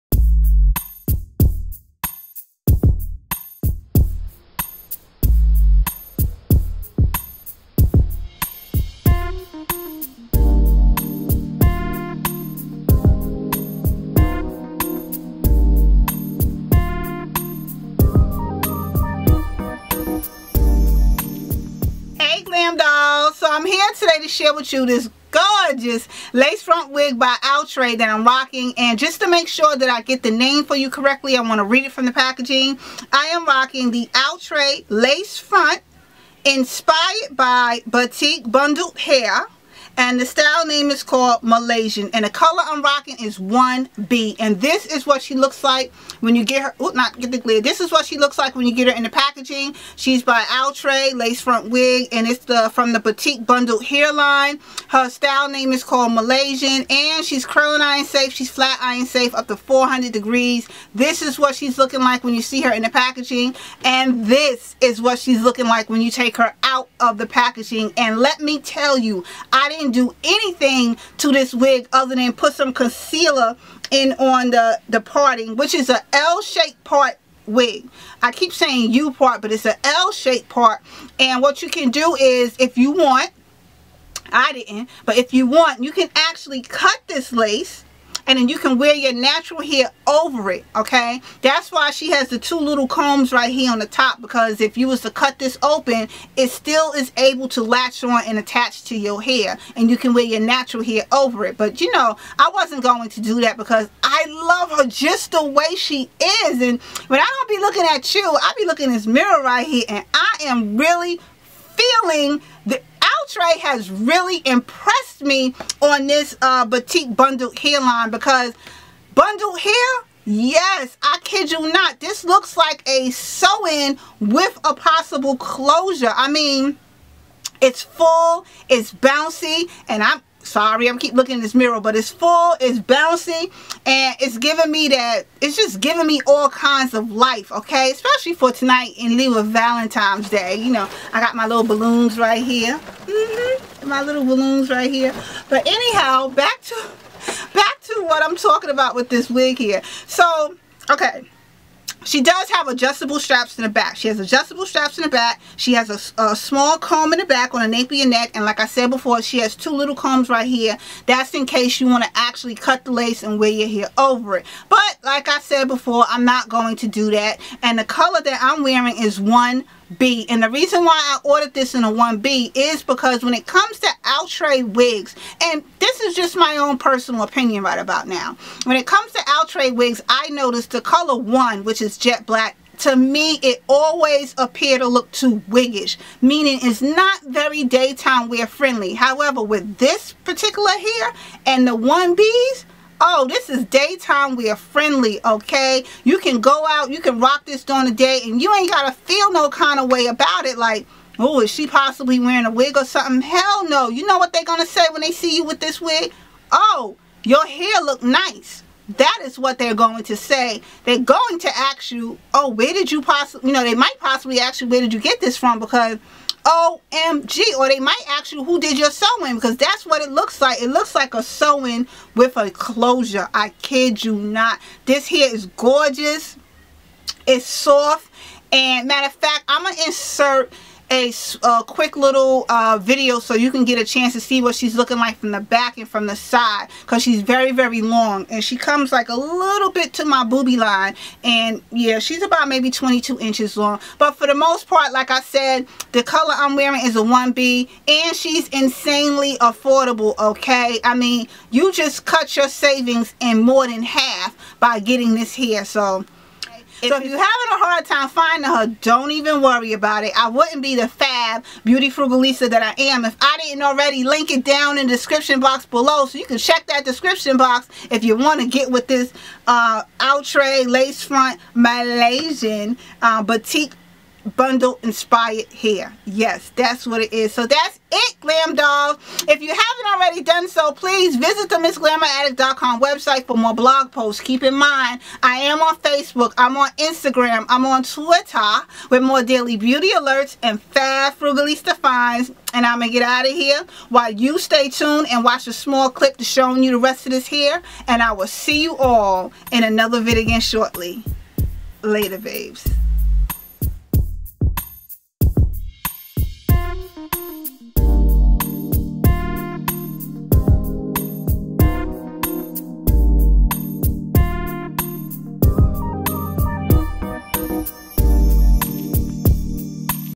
Outre "MALAYSIAN" Batik Bundled Hair Lace Front Wig (color 1B)
For more blog posts.

You can purchase the wig here:
Hair: Outre “Batik Bundle Hair” Lace Front Wig “Malaysian” Color 1B

Do You Like My Makeup?

Product Details:

I'm an everyday lady who like sharing my beauty finds and was pleasantly surprised when i received this Outre "MALAYSIAN" Batik Bundled Lace Front Wig and it was actually better than pictured when taken out the package..

I am a true believer in a company keeping its word and delivering its promise to what is printed or shown on their labels and the Outre "MALAYSIAN" Batik Bundled Lace Front Wig SUPER EXCEEDED my expectations.

The Outre "MALAYSIAN" Batik Bundled Lace Front Wig is easy to manintain, its curling iron safe up to 400 degrees and is made from 100% premium fiber. I must say it sure did exceed my expectations, which is why I am uploading this review.

Outre "MALAYSIAN" Batik Bundled Lace Front Wig is super affordable and the quality of the hair is the same as the higher end brands of bundled hair but at a fraction of its cost.

I am a true believer in being a BeautyFrugalista and love knowing I don’t have to break my bank for expensive hair when I can get the same quality at a more affordable price.

Thank you for taking the time to watch my video, it is appreciated!

I have not been compensated for my posting my video nor was I given any product free in exchange for showing how i used it. I purchased the items mentioned & shown with my own money.